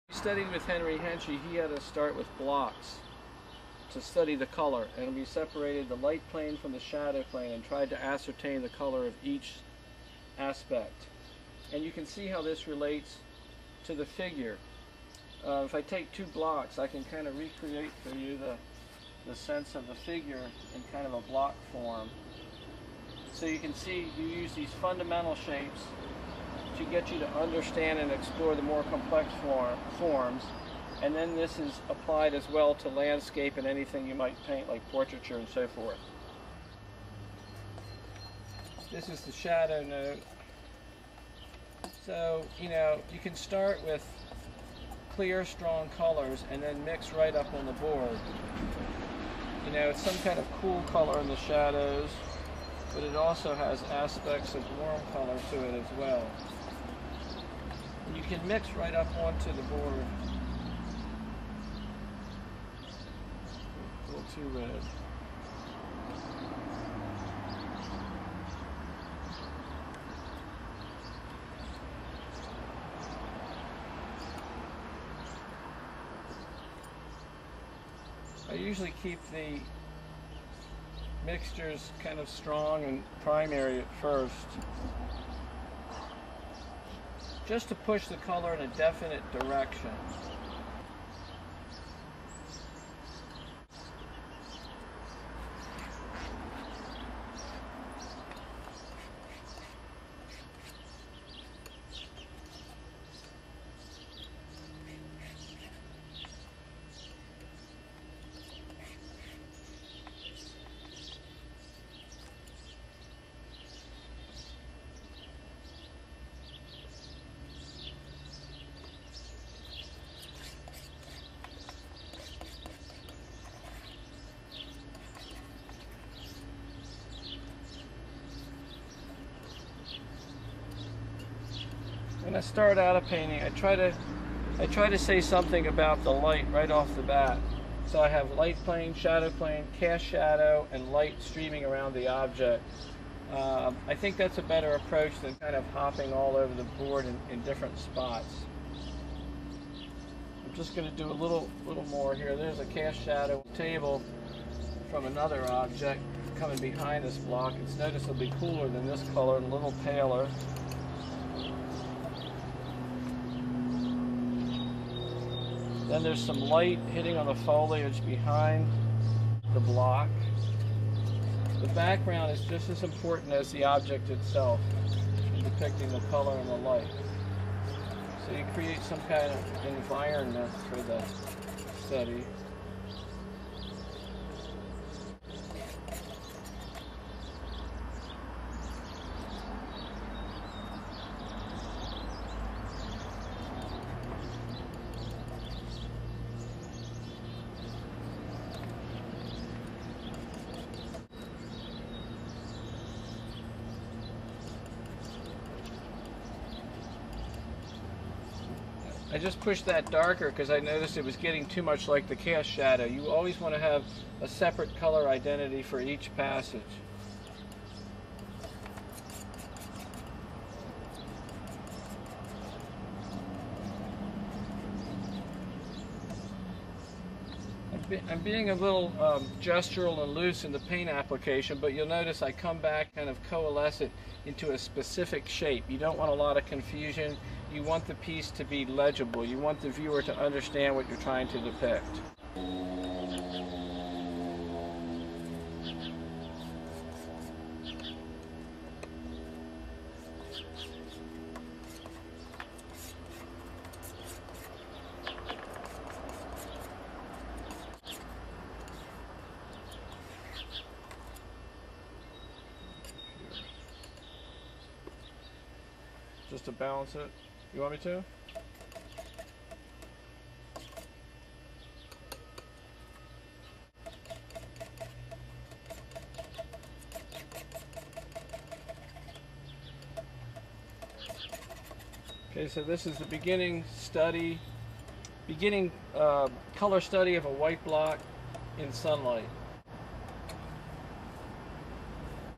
The White Block in Sun Light
John Ebersberger talks  about and demonstrates Painting the White Block in Sunlight.